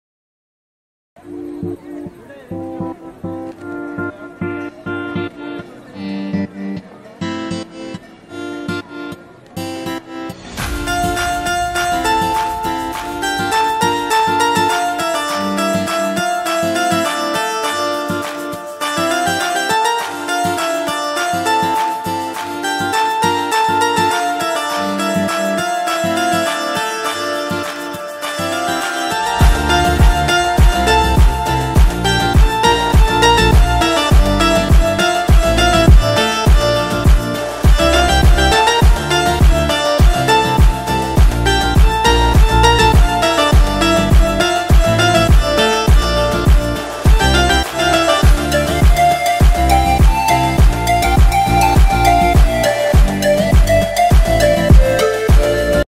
Latest Kundan Wedding Jewelery Wholesale | Choker Dulhan Set Sadar Delhi
All Type of Premium & Fancy Kundan Jewellery available for party & wedding functions
Bridal Special Collection is also Available

Shagun Jewellers
5163 , Rui Mandi, Sadar Bazar ,
Delhi - 110006